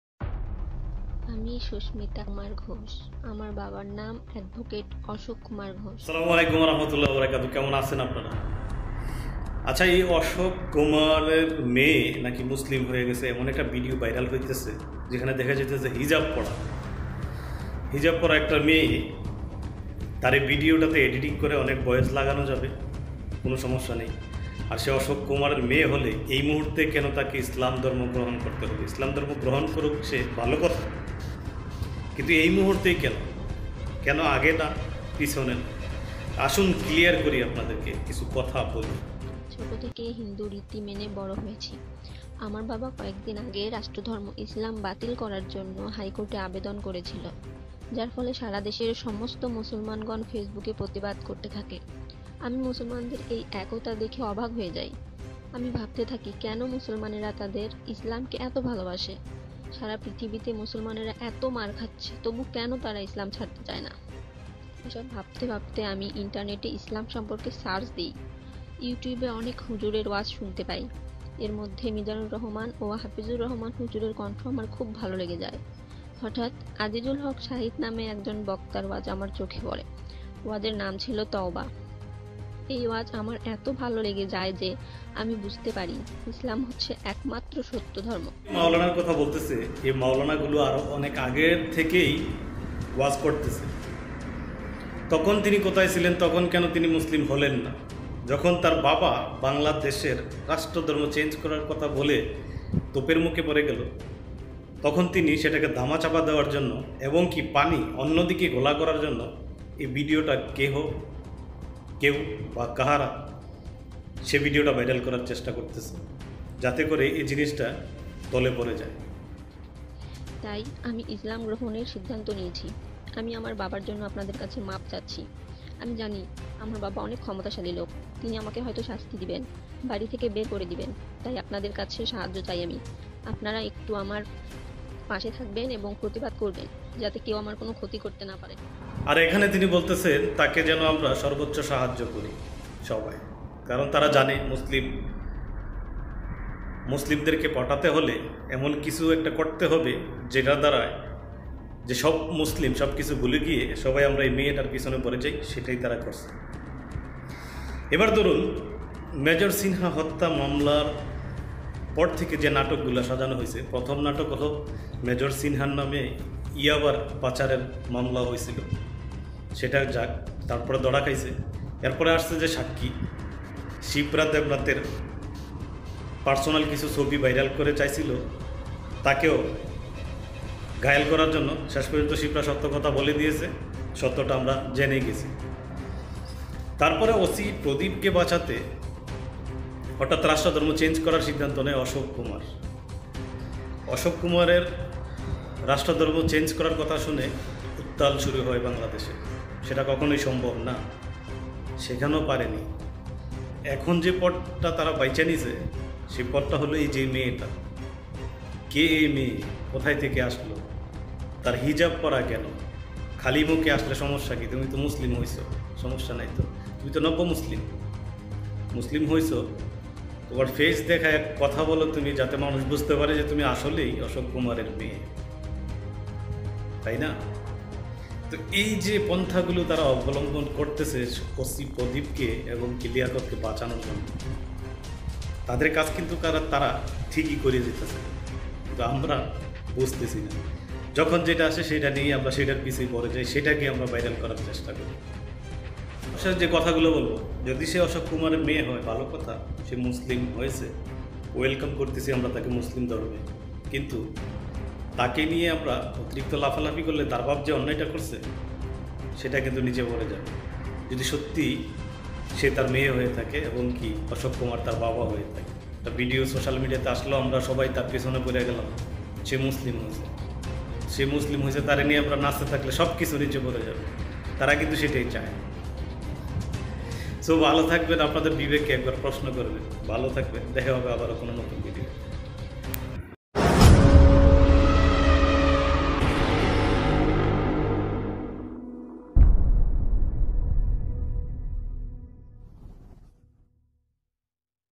অশোক কুমারের মেয়ে মুসলিম হওয়ার ||মুখোশ উন্মোচন||Bashir Ahamad miazi
অশোক কুমারের মেয়ে মুসলিম হওয়ার মুখোশ উন্মোচন করা হচ্ছে ।। এবং এটি আমার ওপেনিয়ন আপনার টা অন্ন ও হতে পারে মাফ করবেন।।